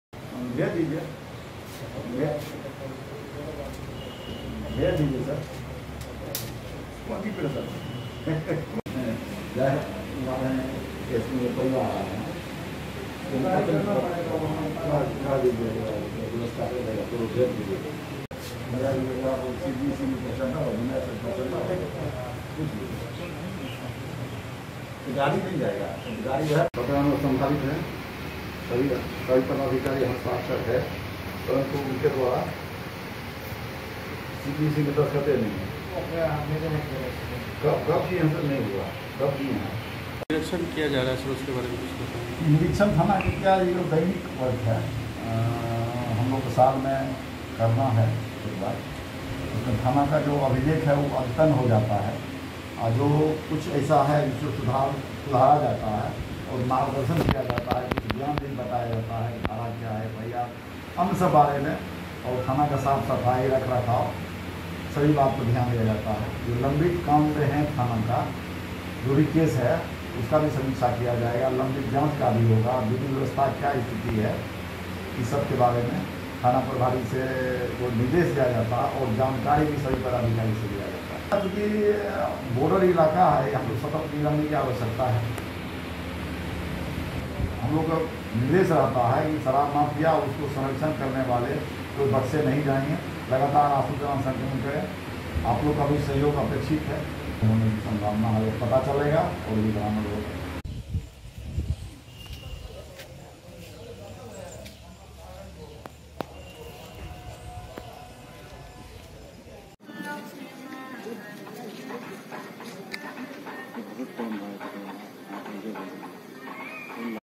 लंबित कांडों का शीघ्र करें निष्पादन: एसडीपीओ, बेनीपट्टी एसडीपीओ ने किया हरलाखी थाने का निरीक्षण...
लंबित कांडों का शीघ्र करें निष्पादन: एसडीपीओ

बेनीपट्टी एसडीपीओ ने किया हरलाखी थाने का निरीक्षण

संवाददाता
हरलाखी (मधुबनी)

बेनीपट्टी एसडीपीओ अरुण कुमार सिंह ने रविवार को हरलाखी थाने का निरीक्षण किया। निरक्षण के दौरान उन्होंने अभिलेख पंजी, स्टेशन डायरी, हाजत पंजी, मालखाना पंजी, फरारी पंजी व सिरिस्ता के अलावा अन्य पंजियों निरक्षण के साथ विभिन्न कांडों की समीक्षा भी किया। उन्होंने लंबित कांडों के निष्पादन मे तेजी लाने व वारंट पेंडिंग को लेकर सख्त निर्देश थानाध्यक्ष व आईओ को दिया। थाना क्षेत्र में नियमित रूप से गश्ती करने व कांड में फरार अभियुक्तों की गिरफ्तारी अभियान चला कर गिरफ्तारी सुनिश्चित करने का भी दिशानिर्देश दिया। वहीं शराब माफियाओं के विरुद्ध छापेमारी करने के साथ ही सघन वाहन जांच अभियान चलाने के साथ असमाजिक तत्वों पर विशेष नजर बनाए रखने की हिदायत दी। उन्होंने पत्रकारों को संबोधित करते हुए कहा कि थाना में प्रायः अभिलेख अधतन हो जाता है। जिसको लेकर हर वर्ष थाने का निरीक्षण कर सभी कांडों की समीक्षा की जाती है। साथ ही अगर कहीं कोई कमी रहती है तो थानाध्यक्ष को दिशानिर्देश व मार्गदर्शन भी दिया जाता है।

उन्होंने यह भी कहा कि शराब माफिया व उनको प्राश्रय देने वाले को किसी भी हाल में बख्सा नही जायेगा। शांति व सुरक्षा पुलिस की पहली प्राथमिकता है।

मौके पर थानाध्यक्ष प्रेम लाल पासवान, एसआई धनंजय सिंह, एएसआई नीरज चौबे, एएसआई धर्मेंद्र कुमार, एएसआई आरपी यादव, वीरेंद्र कुमार, शिवशंकर पासवान समेत दर्जनों पुलिस कर्मी व चौकीदार भी मौजूद थे।